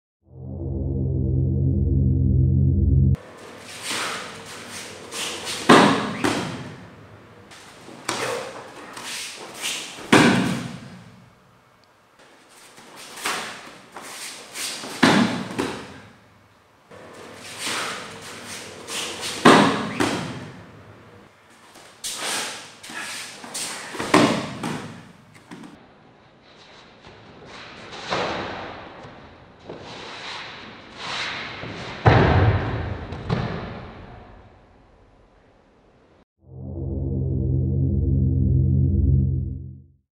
Ken nagashi no kata, Shoden level, Takagi Yoshin Ryu
Yossi Sheriff, the Akban Academy founder, and Dr. Dan Hashimshoni, document Ken nagashi, a Shoden level Kata from Takagi Yoshin Ryu for the AKBAN Academy project.